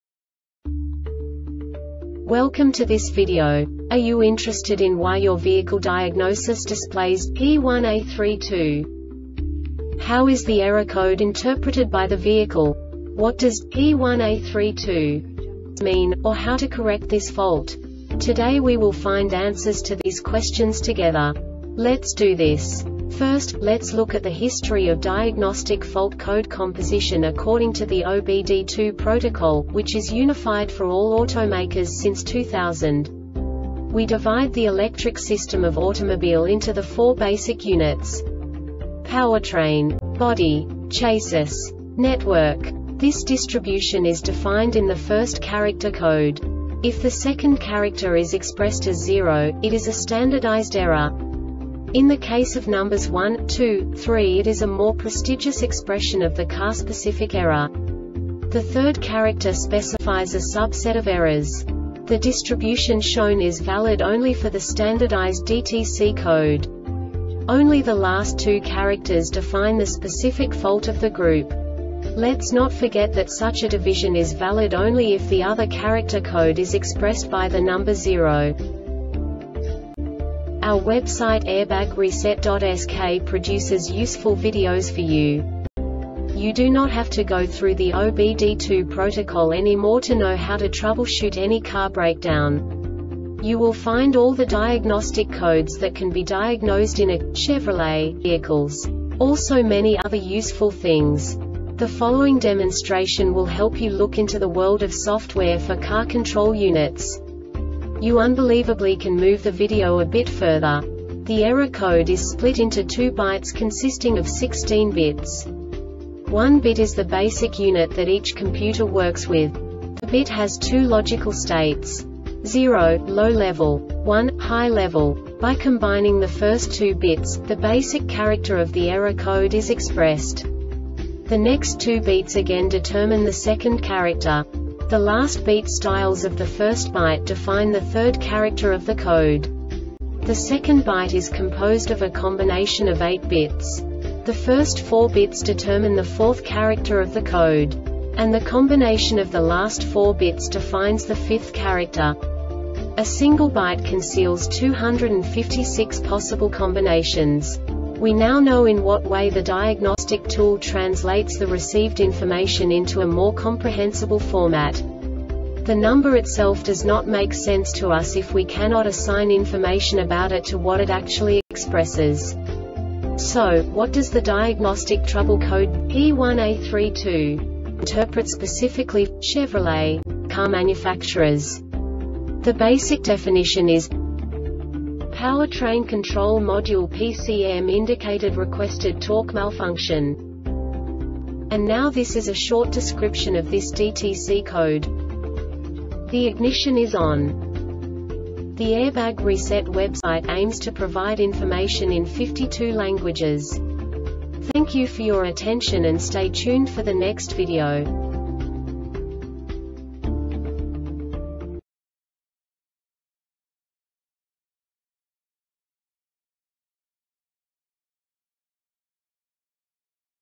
DTC Chevrolet P1A32 Short Explanation
Contents:
0:21 Basic DTC analysis according to OBD2 protocol standard.
1:48 Insight into programming
2:58 A diagnostically understandable description of the Diagnostic Trouble Code
3:05 Basic error definition

Make:
Chevrolet
Code:
P1A32
Definition:
Powertrain Control Module (PCM) Indicated Requested Torque Malfunction
Description: